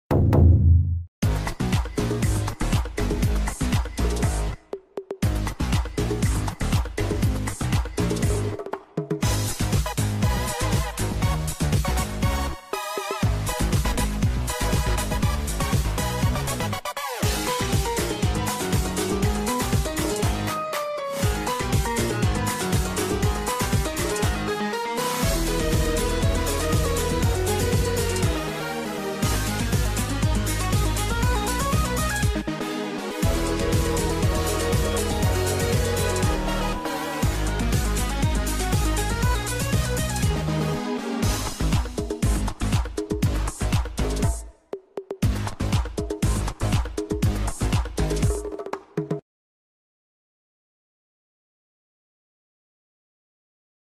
Dancing Marbles Marble Race In Algodoo
We mixed Algodoo with rhythm.
Do you like this style?